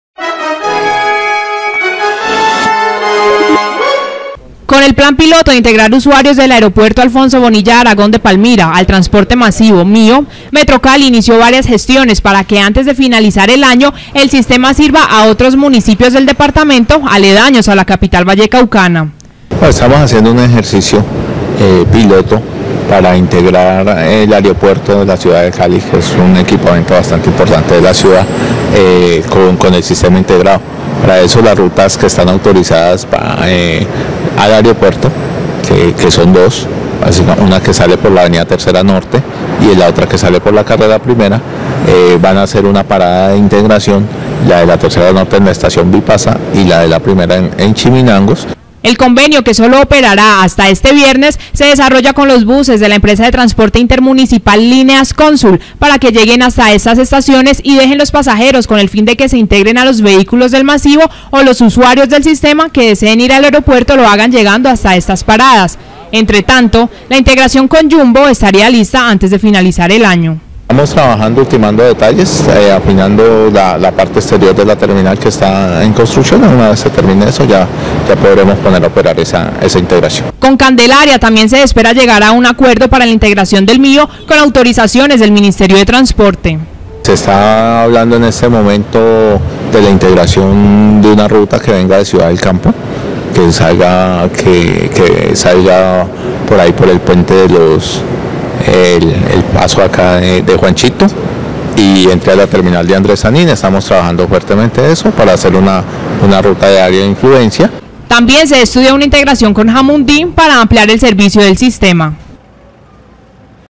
Empezó la integración del MIO con otros municipios
Con el plan piloto de integrar usuarios del Aeropuerto Alfonso Bonilla Aragón de Palmira, al Transporte Masivo, MIO, Metrocali inició varias gestiones para que antes de finalizar el año, el sistema sirva a otros municipios del departamento, aledaños a la capital vallecaucana.

El convenio que solo operará hasta este viernes se desarrolla con los buses de la empresa de transporte intermunicipal Líneas Cónsul para que lleguen hasta estas estaciones y dejen los pasajeros con el fin de que se integren a los vehículos del masivo, o los usuarios del sistema que deseen ir al aeropuerto lo hagan llegando hasta estas paradas. Entre tanto, la integración con Yumbo estaría lista antes de finalizar el año.

Con Candelaria también se espera llegar a un acuerdo para la integración del MIO, con autorizaciones del Ministerio de Transporte.

También se estudia una integración con Jamundí, para ampliar el servicio del sistema.